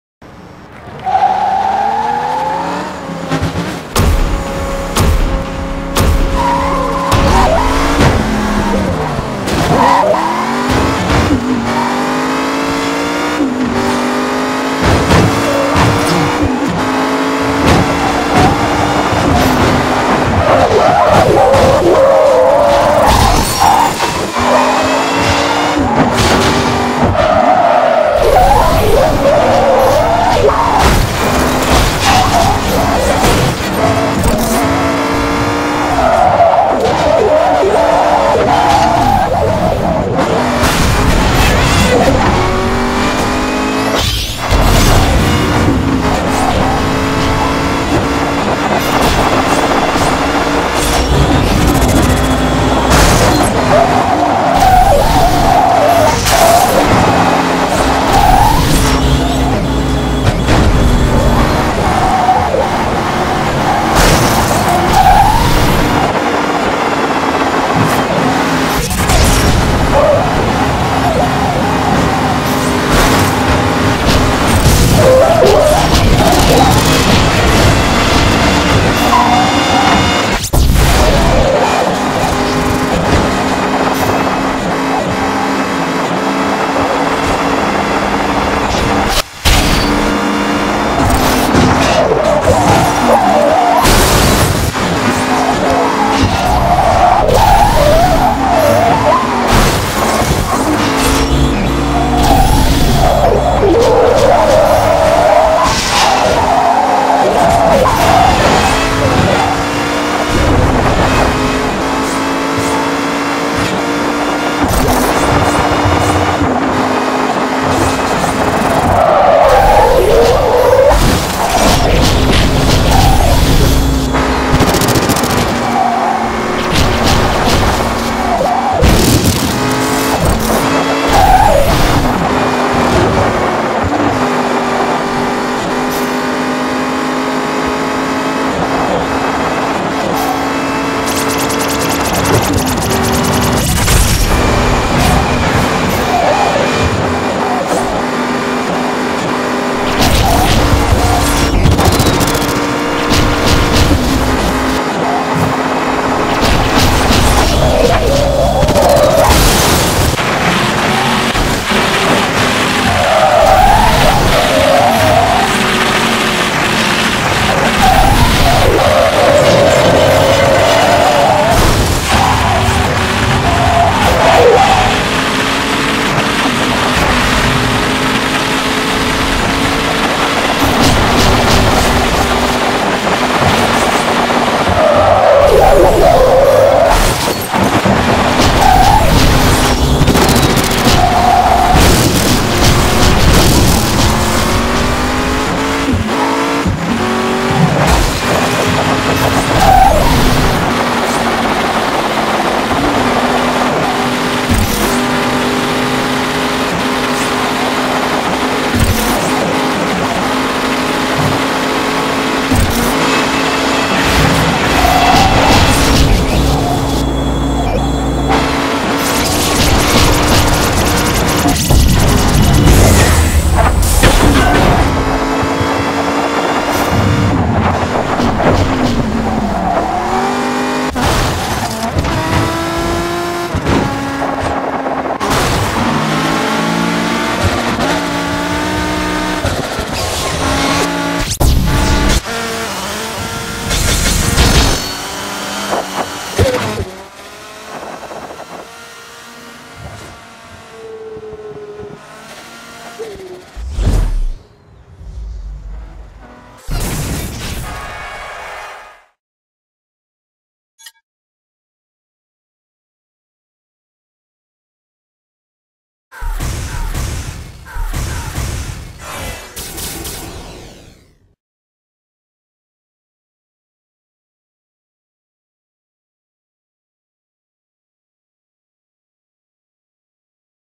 my 1st win with a busted joy pad 28th dec 2012
hi all,one more joy pad busted and its a lobby from hell,so shocks too,pls enjoy this was my 1st win with the "i dont want too work joy pad"se u on track laters;-))